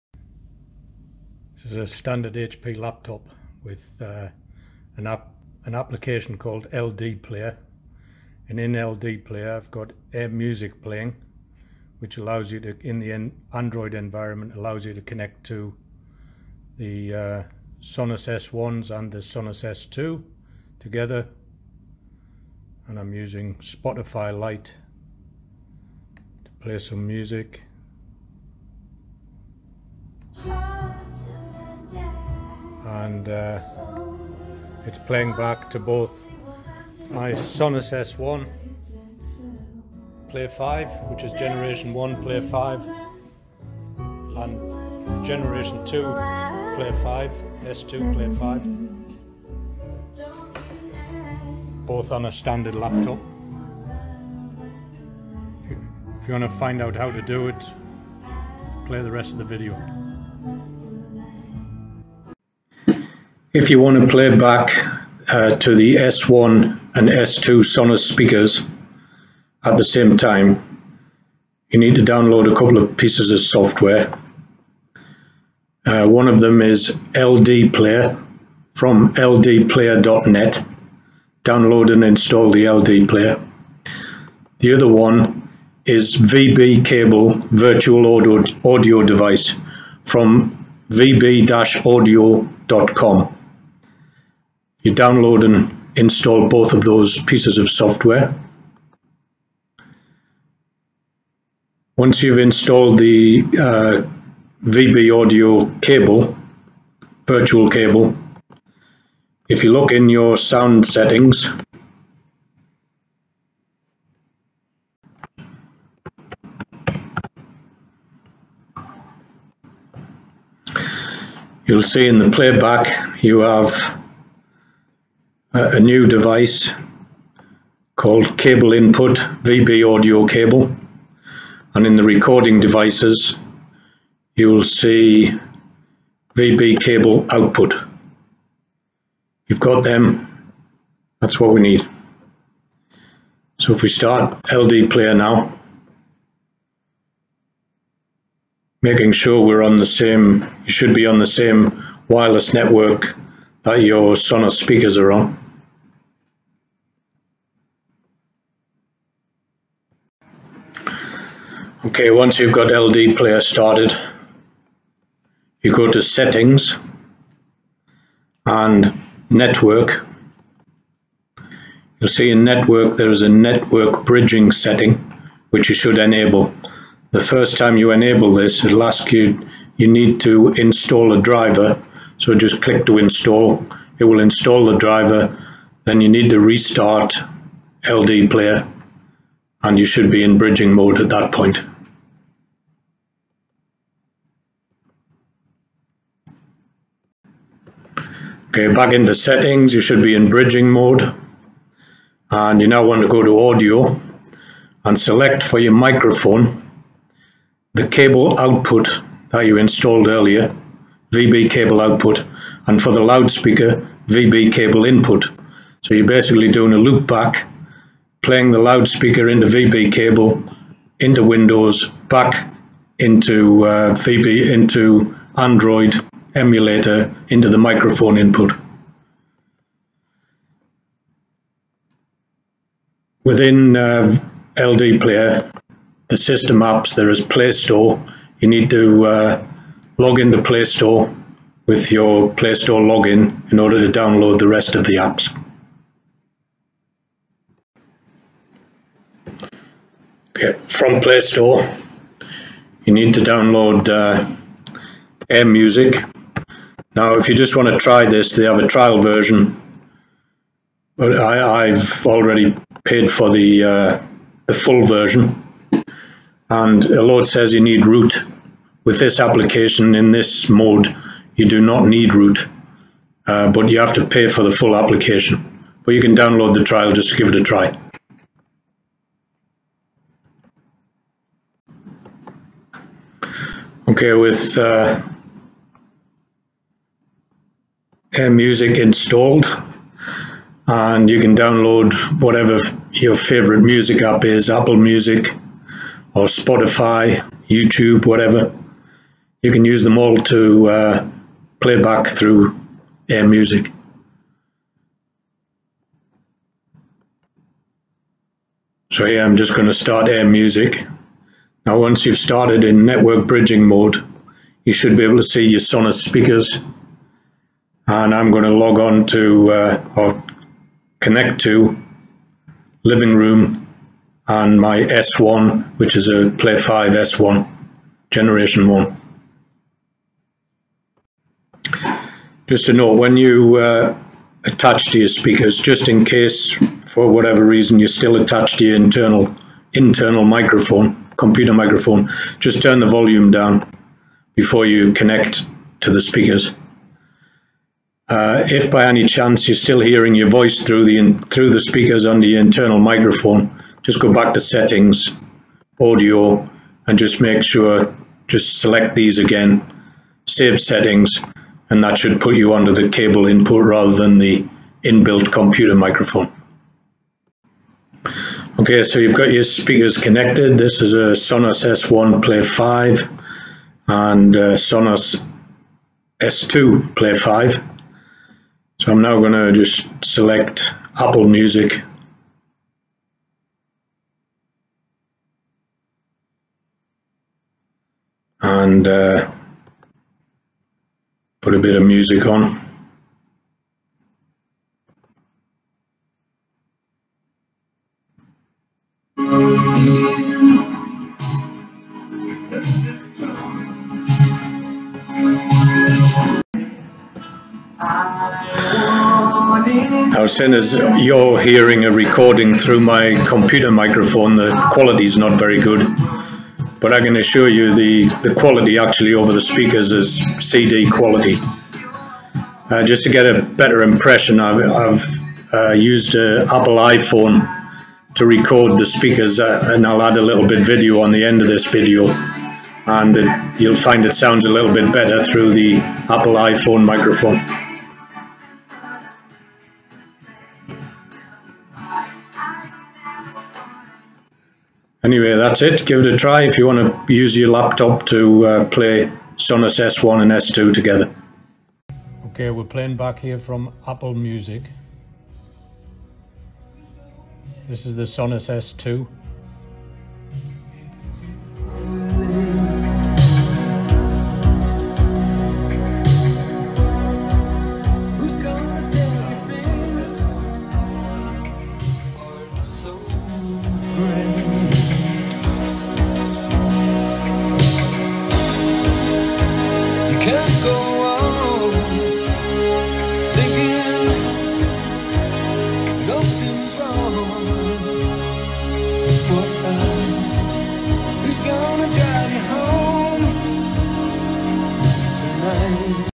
Play Sonos S1 and S2 together using a PC or Laptop
How to Play Sonos S1 (generation1) and Sonos S2 (generation2) speakers together in synch on a standard laptop or PC. Note PC must be capable of virtualization (less than 5 years old)  core I5 or I7.

Download and install LDplayer from LDPLAYER.NET.  I used LDPLAYER as this emulator allows stereo sound input the other emulators only have Mono microphone input. This is an android emulator which allows you to run android apps on a PC

Download VB virtual cable from VB-AUDIO.com and install as described on their website.